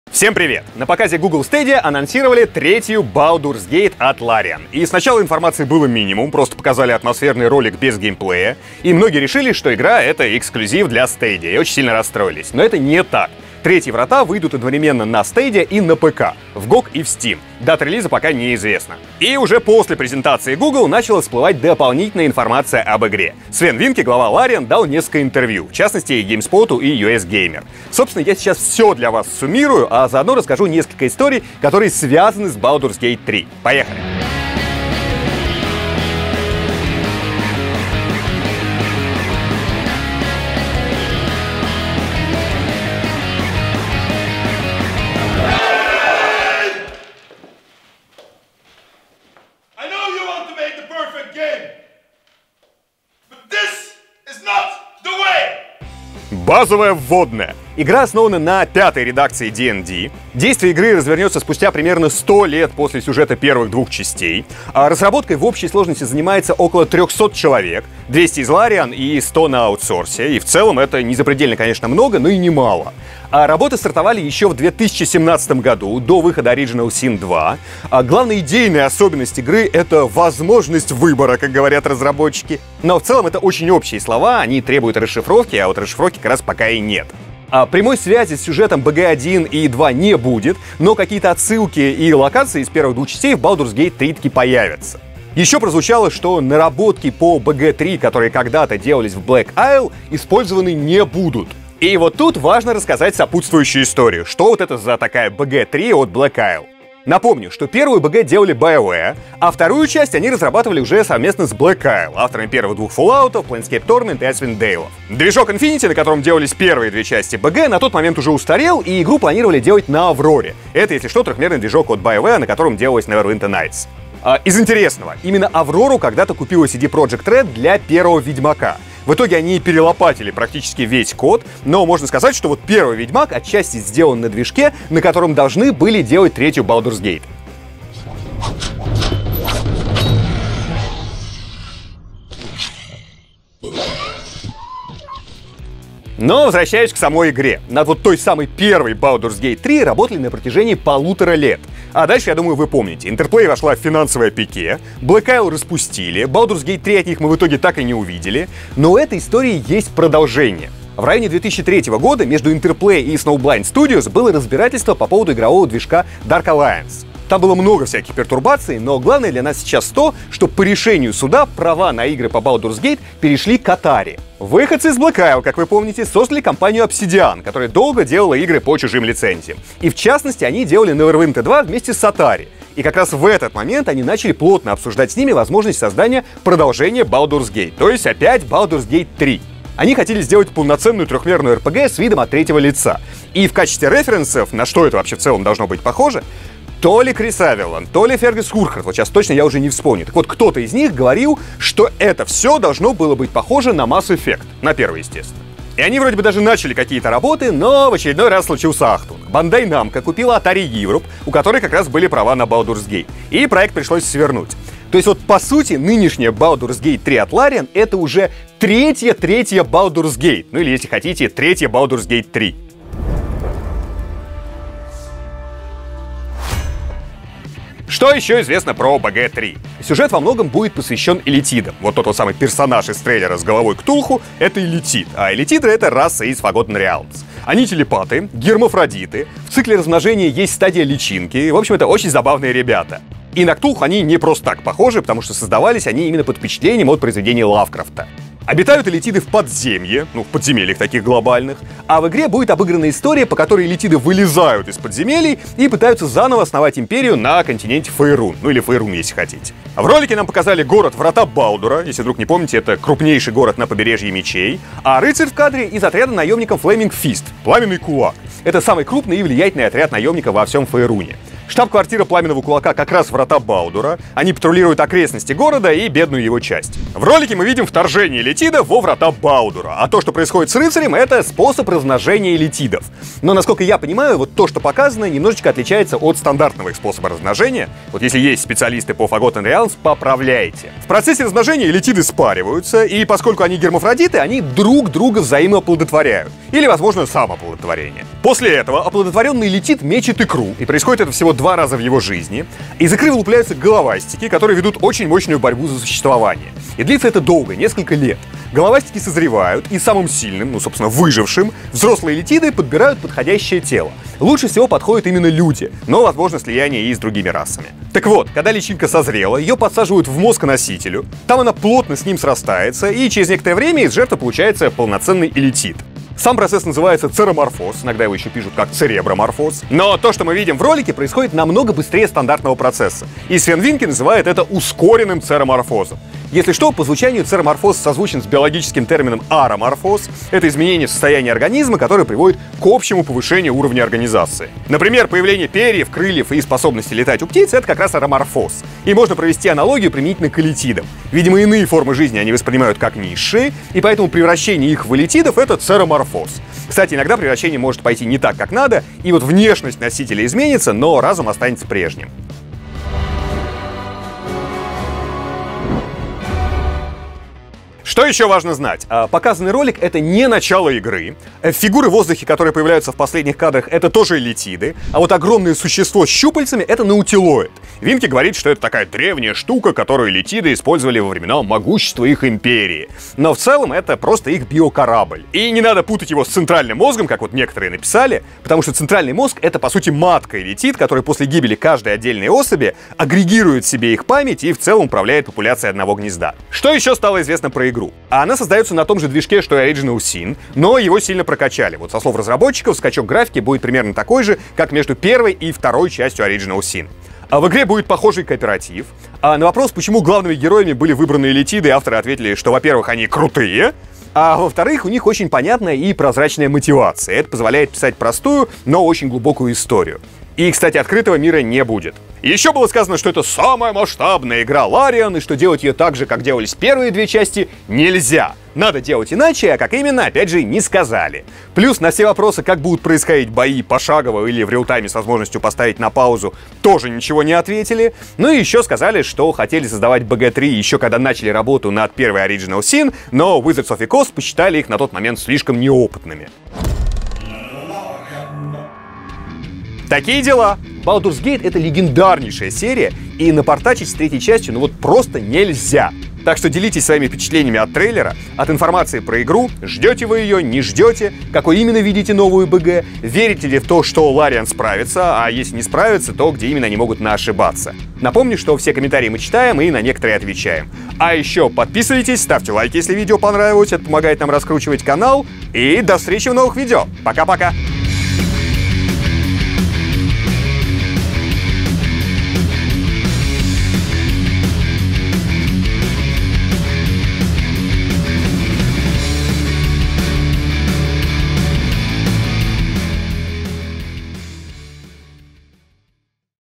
Все подробности о Baldur's Gate 3
Larian Studios анонсировали Baldur's Gate 3 - продолжение легендарнейшей серии RPG. Алексей Макаренков прошерстил все интервью разработчиков, внимательнейшим образом изучил трейлер, собрал воедино всю информацию - и теперь готов поделиться всем этим с вами. Что ждет нас в третьих "Вратах Балдура", как размножаются иллитиды, почему третья Baldur's Gate уже третья по счету… все это в нашем материале.